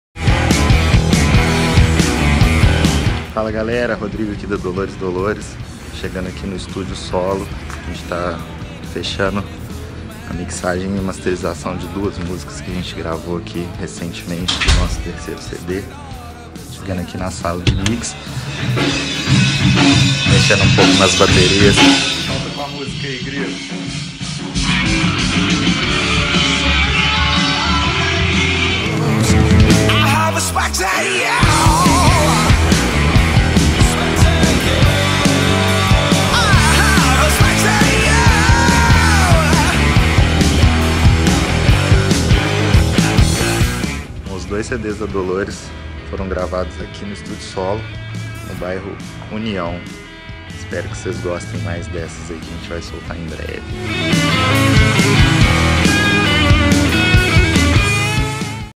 Dolores Dolores - Mixagem e Masterização
Um dos mais renomados estúdios do Brasil, localizado em Belo Horizonte/MG no bairro União.

Fechando a Mixagem e Masterização de 2 novos Singles da Dolores Dolores, novamente a cargo do Rodrigo Aires Grilo, com produção de Humberto Maldonado.

Confira Agora